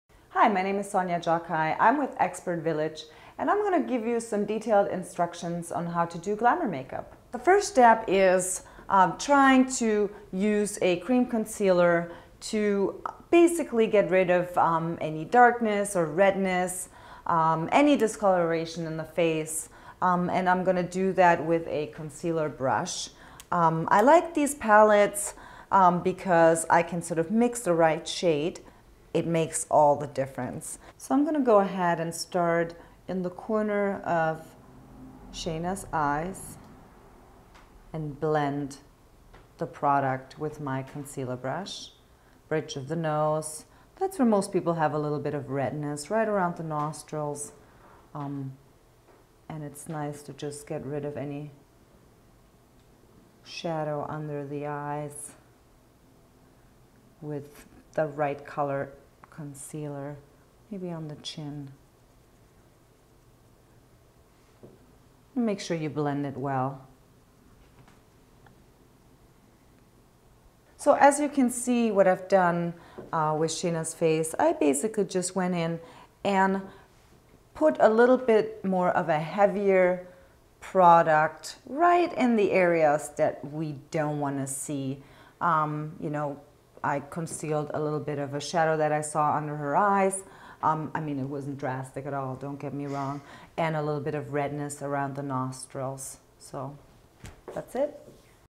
How to Put on Makeup : Applying Concealer for Smoky Eye Look
Applying concealer will help set the stage for smoky eyes.  Learn how to apply concealer in this free makeup video.

Expert: Sonja Gjokaj
Bio: Sonja Gjokaj is a Boston-based makeup artist with experience in makeup for production assignments as well as individual on-site makeup services.
Filmmaker: David Jackel